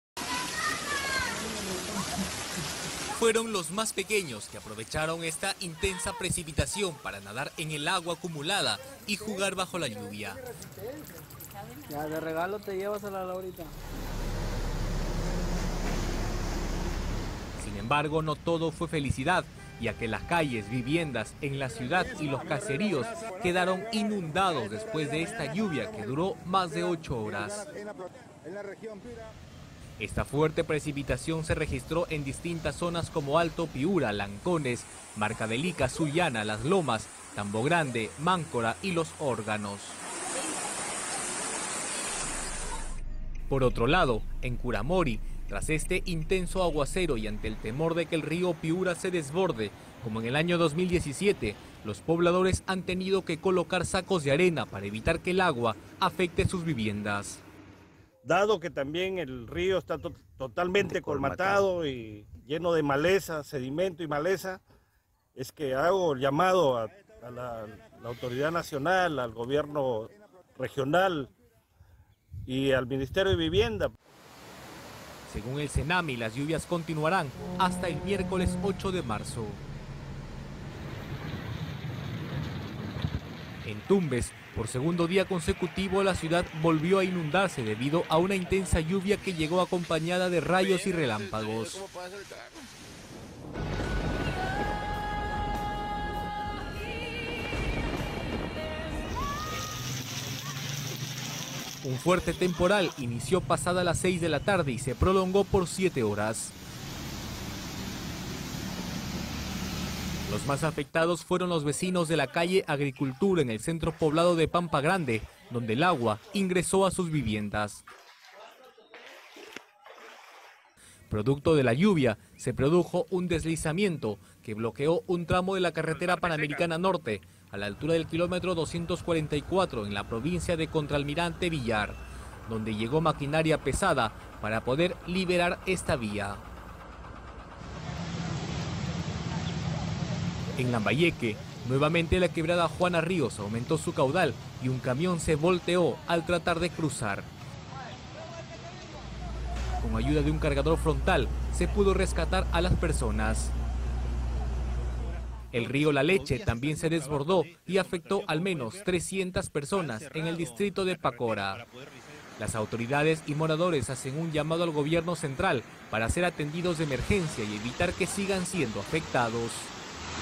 Calles de Piura, Tumbes y Lambayeque quedaron inundadas por intensas lluvias y desborde de ríos
Las intensas lluvias en el norte del país continúan. Las regiones de Piura, Tumbes y Lambayeque registraron precipitaciones que duraron hasta 6 horas. El nivel del agua en las calles inundadas alcanzaron hasta los 40 centímetros de alto. Los vecinos se mostraron muy alarmados.

Lambayeque

Una quebrada en el distrito Chongoyape, Chiclayo, se volvió a reactivar  como consecuencia de las fuertes lluvias. Por su parte, el río La Leche se desbordó en las últimas horas y afectó a al menos 300 personas en el distrito de Pacora, quienes piden ayuda a las autoridades.

Piura

En este departamento registró una larga e intensa lluvia desde la tarde del lunes 6 de marzo hasta la mañana de este martes. Las calles de al menos 7 ciudades, como Sullana, Yamango y Salitral, fueron inundadas totalmente, decenas de familias fueron afectadas, informa Exitosa.

Tumbes

En esta región se registró fuertes precipitaciones en la tarde y noche del lunes. Las avenidas, calles y los desagües quedaron colapsados luego de que la inundación alcance los 40 centímetros de altura. Damnificados piden ayuda pues perdieron todos sus enseres.